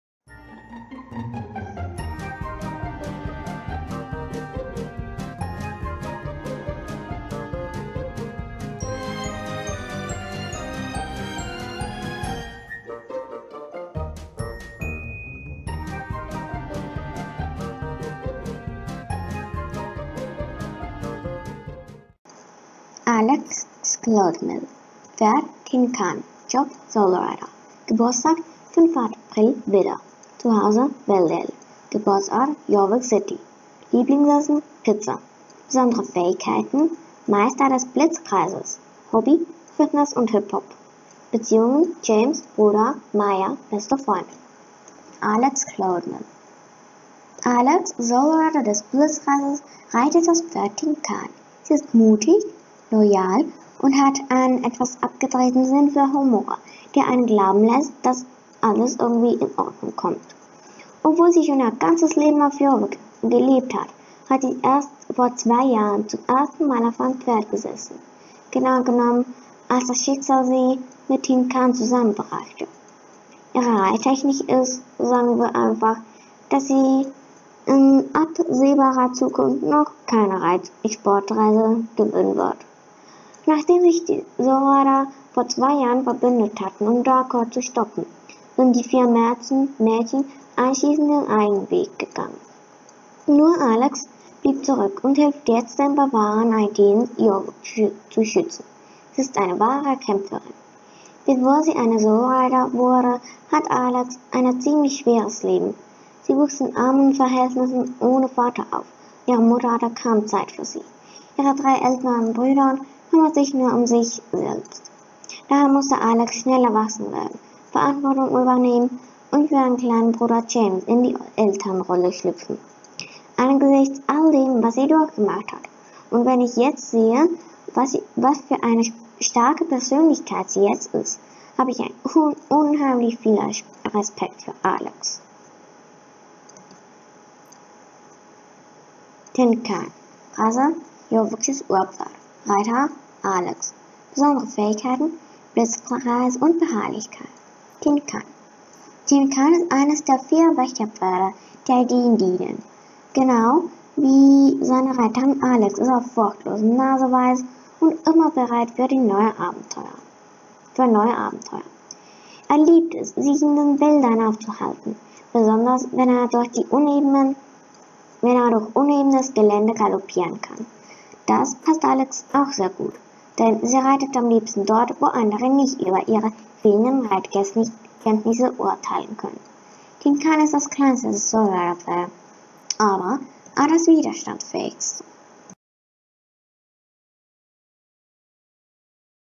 Alex Cloudmill und Tin-Can | SSO
Ich erzähle euch Sachen über Alex und blende euch Bilder von ihr ein.